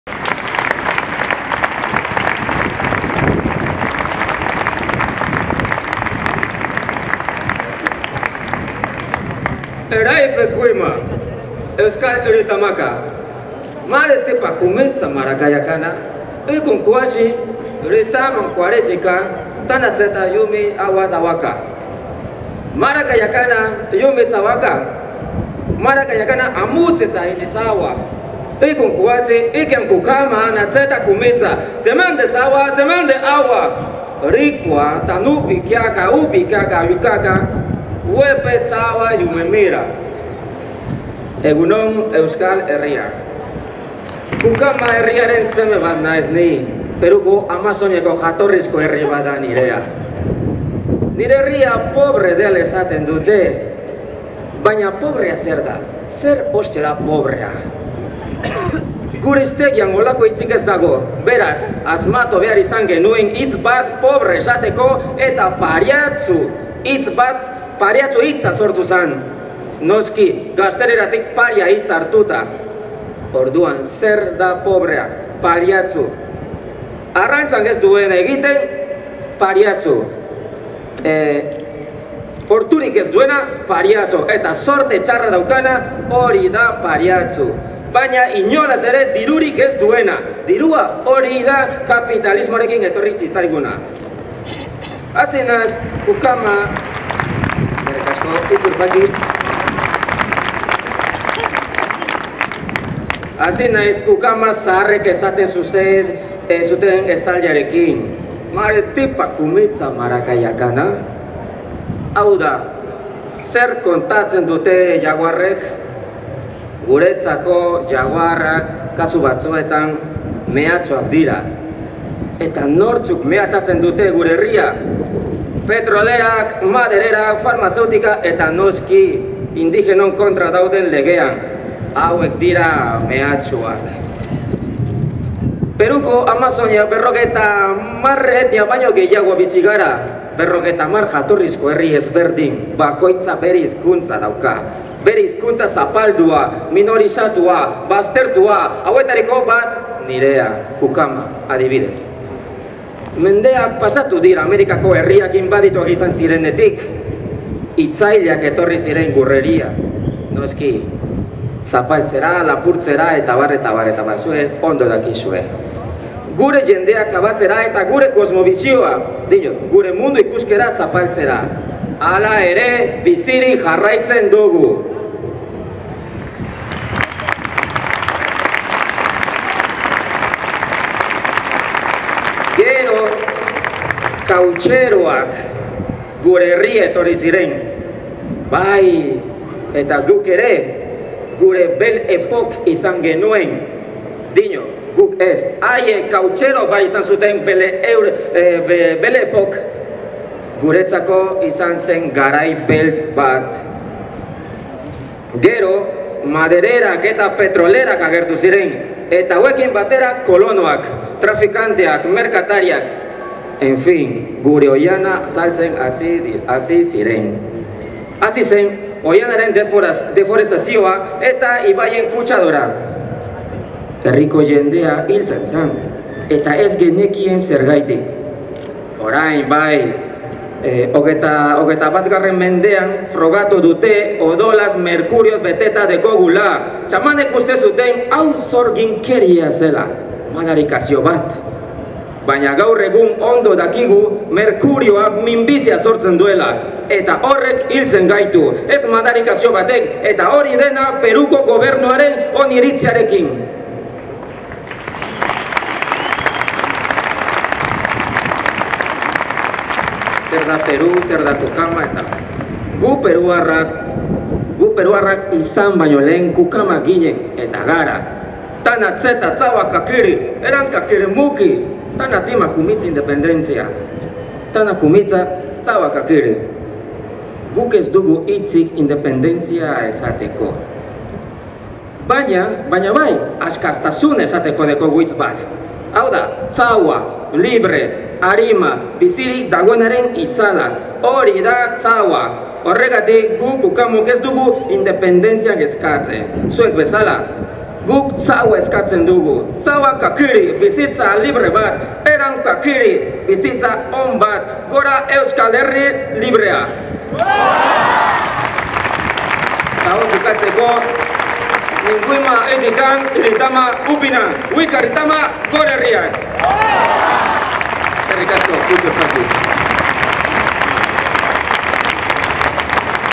AberriEguna2015 Tananta: "Guk bizitza libre bat eskatzen dugu"
Roberto Tananta Awanar gaztearen hitzak Independentistak Sareak deituriko Aberri Eguneko ospakizunetan.
Roberto Tananta Awanari Tupi Guarani amazoniako nazio zabaleko parte den Kukama-Kukamiria herrikoa da. Gaur egun Independentistak Sareko kidea da.